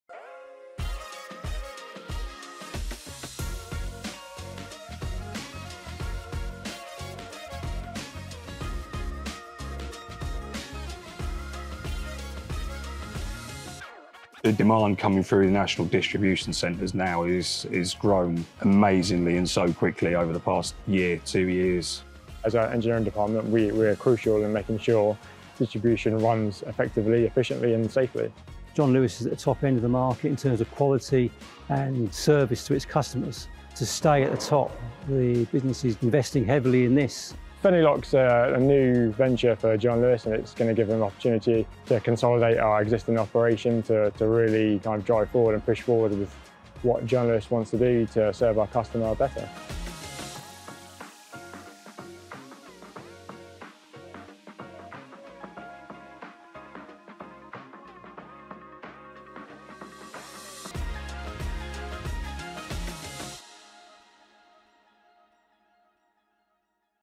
Electrical Engineering - Distribution & Supply Chain
To help our distribution and supply chain running effectively and efficiently, our Electrical Engineers are always on hand. From routine maintenance to raised issues its always a varied day on site.

Hear from our Partners about the role and why they work in the Partnership.